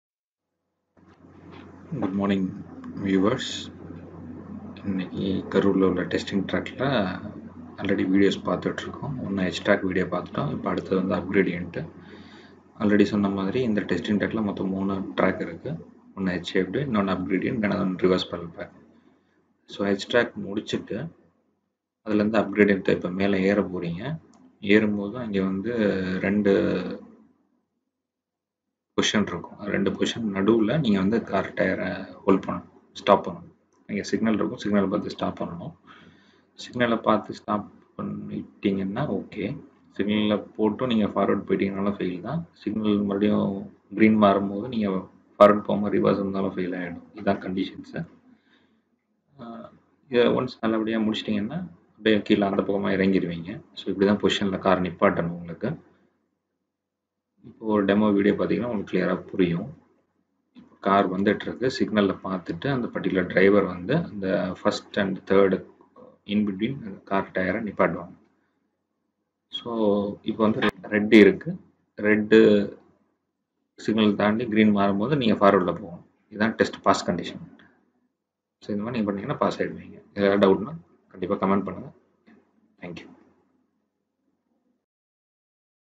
How to pass Up Gradient Test to get LMV License
Karur Regional Transport Office (RTO), has got its first computerized driving testing track consisting of H Track, Up Gradient Track, Reverse Parallel Parking Track Tamil Nadu, developed by M.Kumarasamy College of Engineering, Karur.